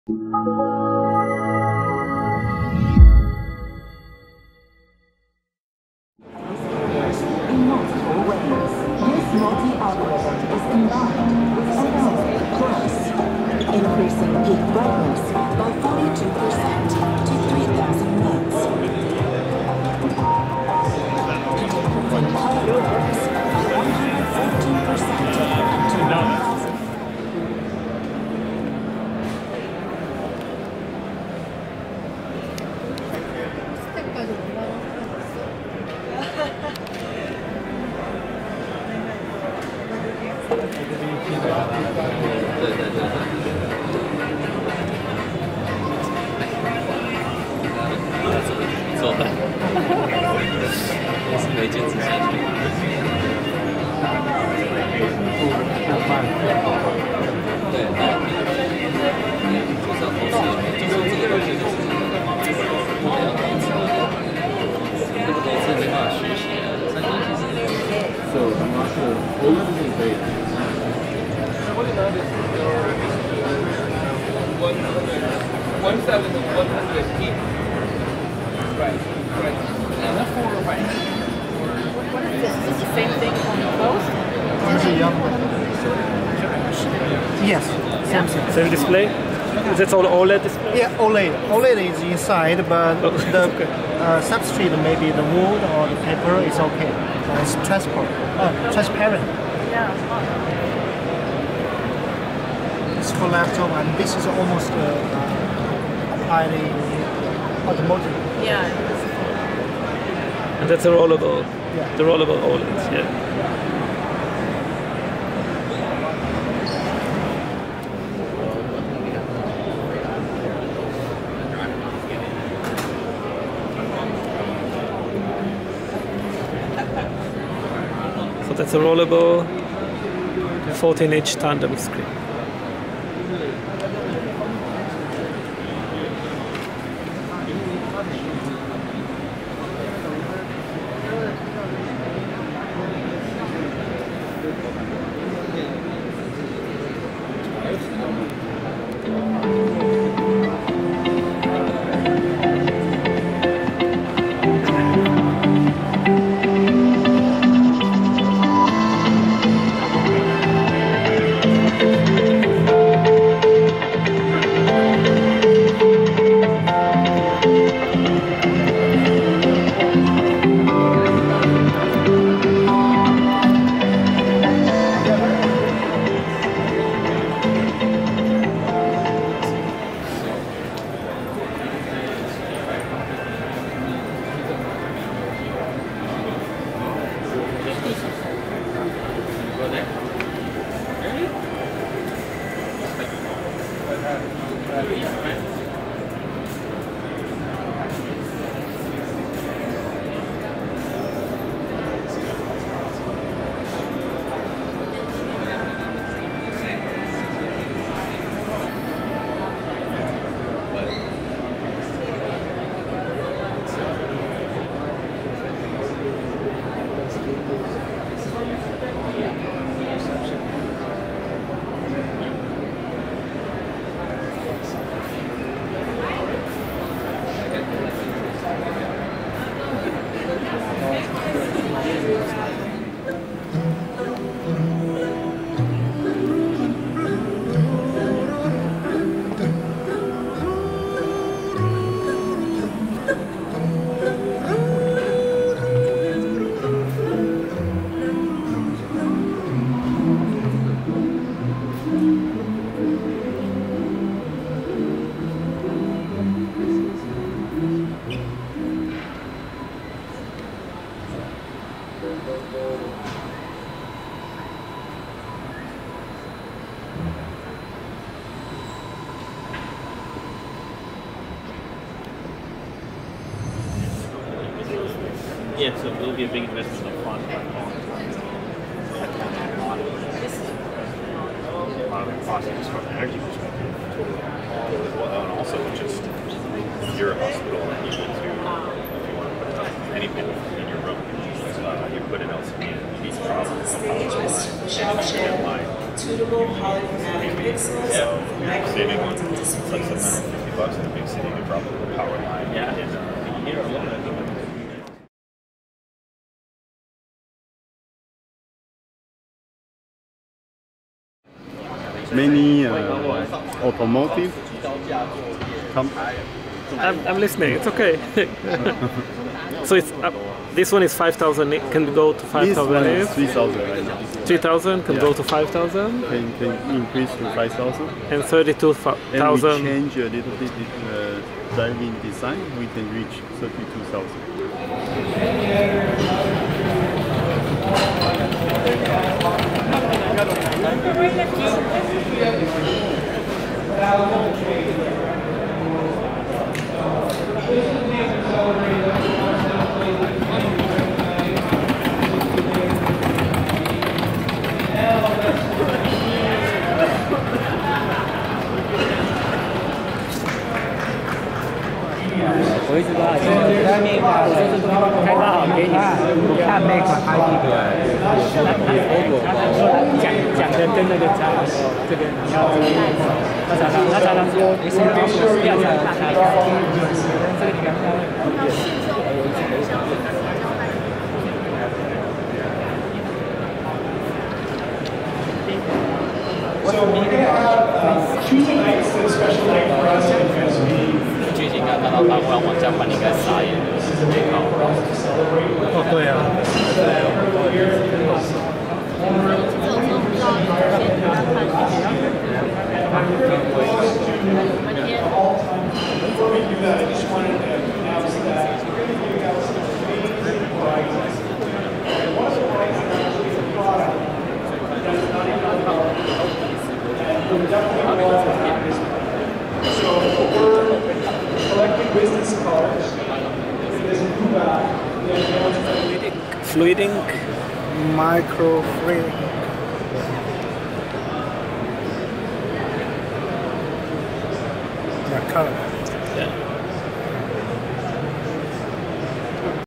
First impressions SID 2024, OLEDs and MicroLED
Here's a short video with some of the displays, mostly OLEDs and microLEDs, shown at Displayweek 2024.